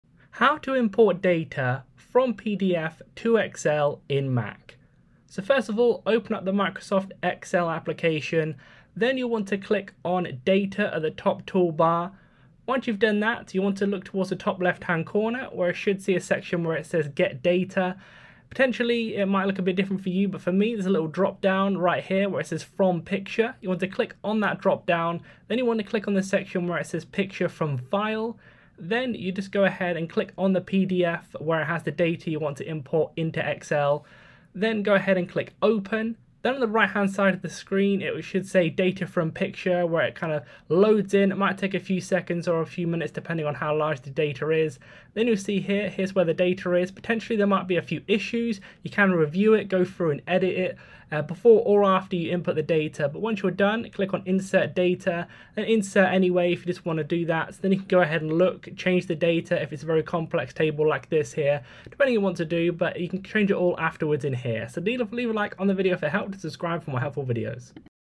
How to Import Data from PDF to Excel on Mac
How to Import Data from PDF to Excel on Mac. Step by step instructions of how to extract data from pdf to excel on mac. This works for microsoft excel and pdf documents on macbook laptops in 2024.